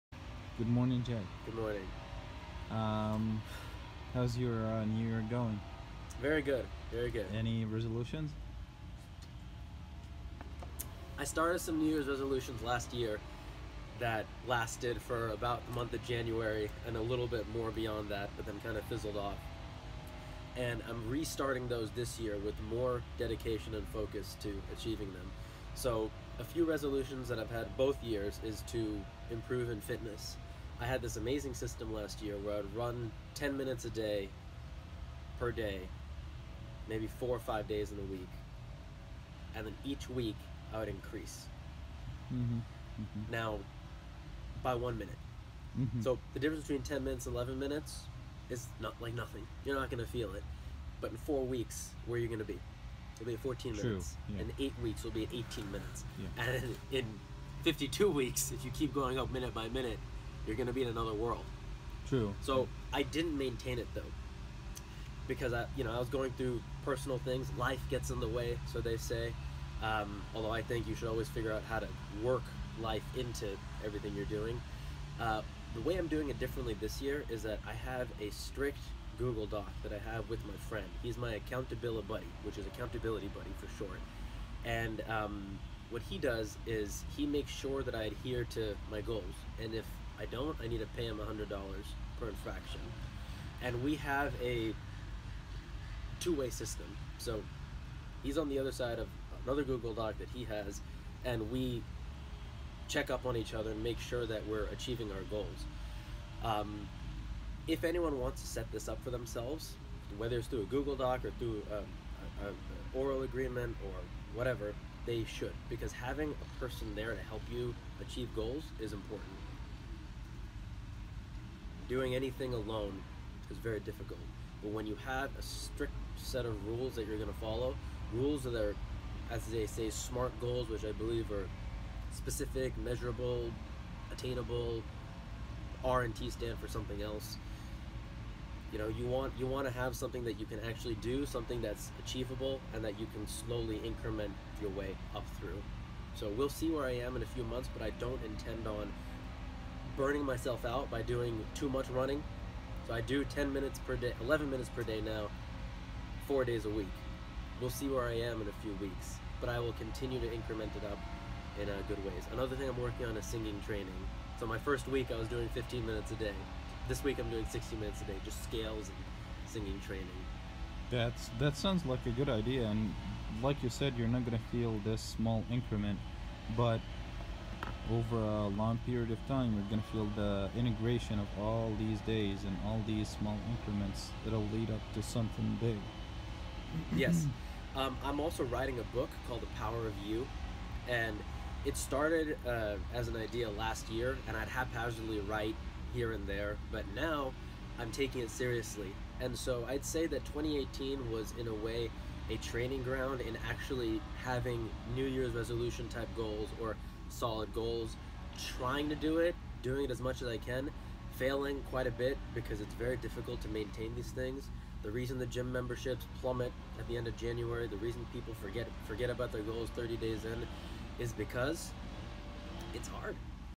My New Year's Resolutions
How 2018 was a building ground for me to properly fulfill New Year's Resolutions and life goals in 2019, and the system and mentalities I use to make these goals from imagination into reality.